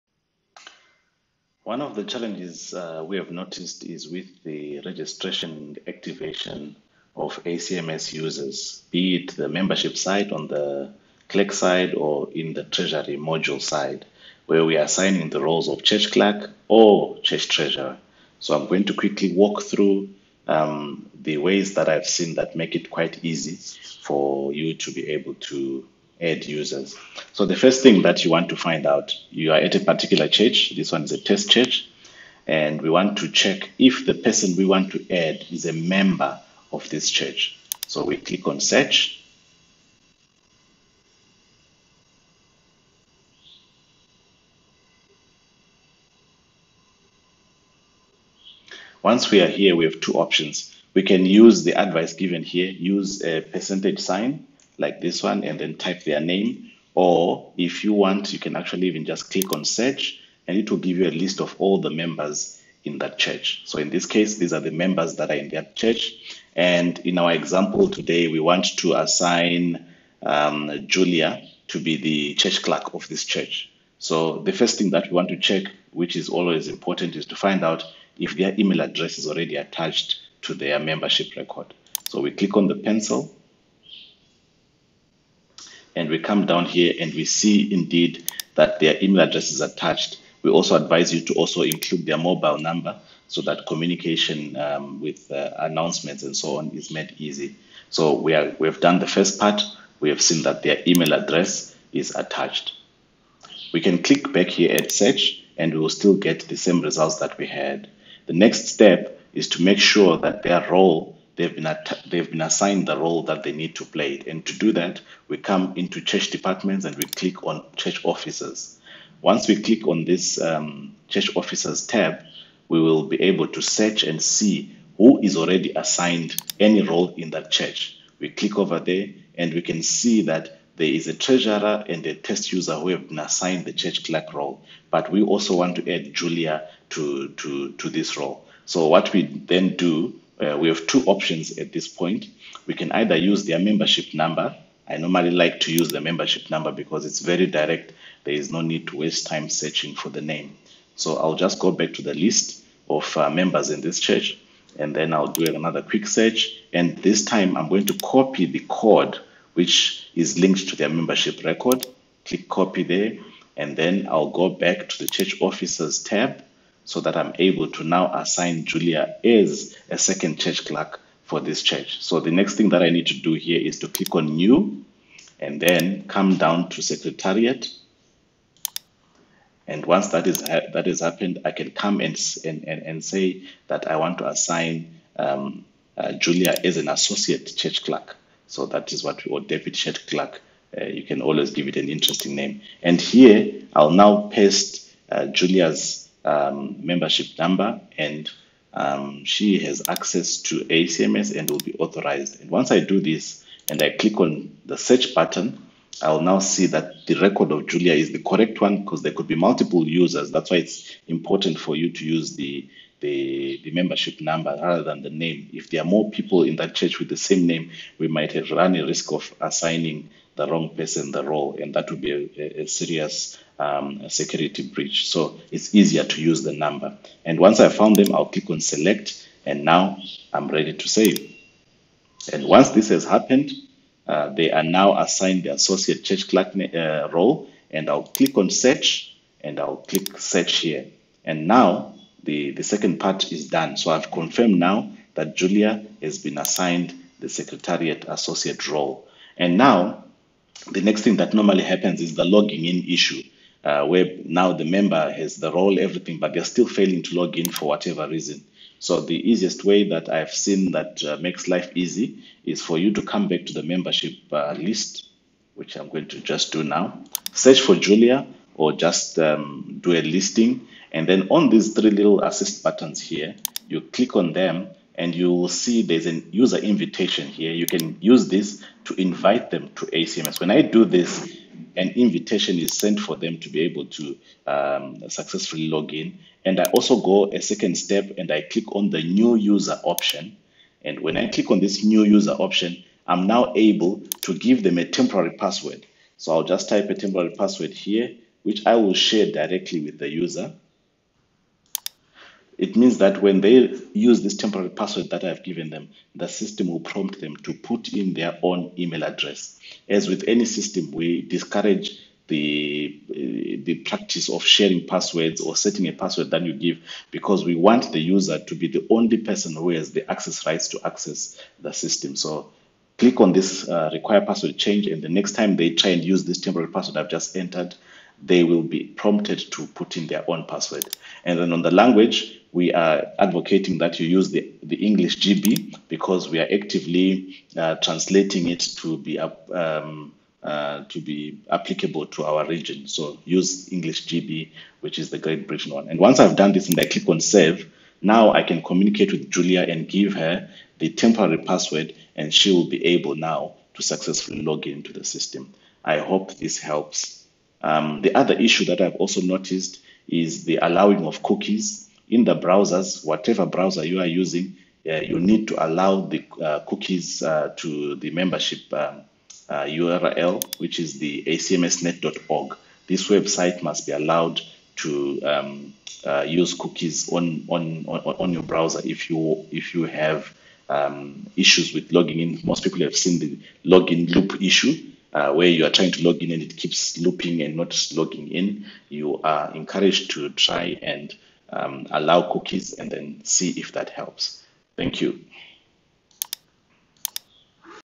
How to solve login issues at Confrence Level
How to help users log into ACMS Treasury system.